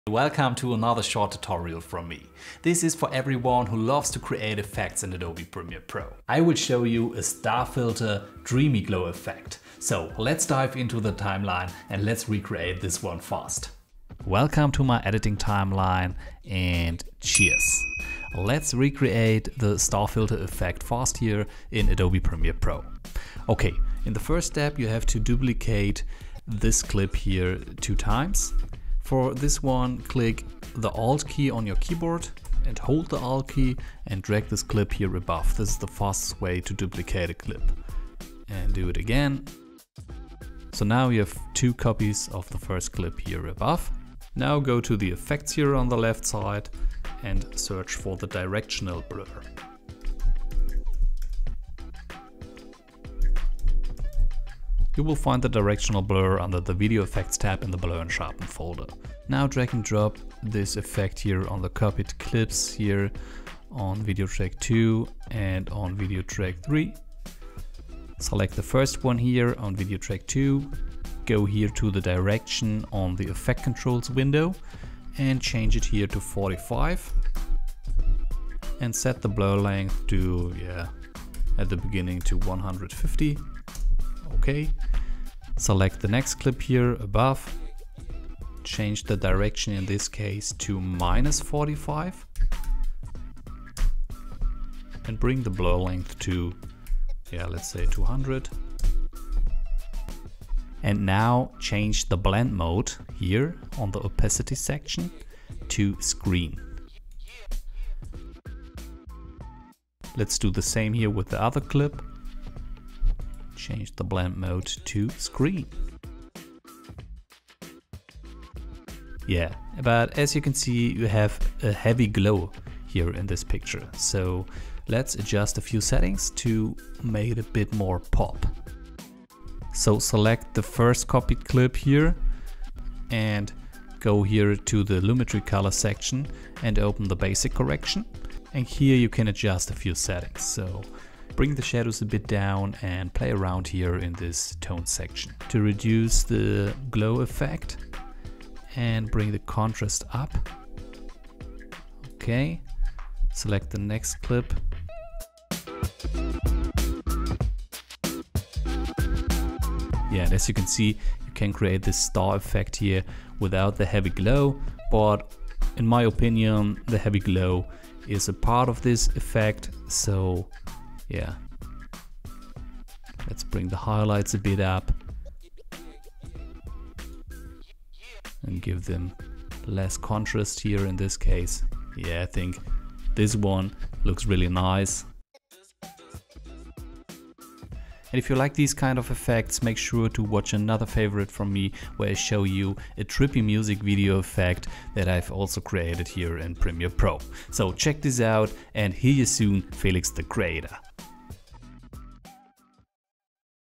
Premiere Pro Glow Effect - STAR EFFECT - Simple, Easy, Fast
Learn how to recreate the Star Glow Effect in Adobe Premiere Pro.
Travel back to the 80's with this one! The Star Glow Effect works nicely in Music Videos or in Movie dream-sequences.

Stockfootage & Music in this video:
The footage used for this clip is from artgrid.io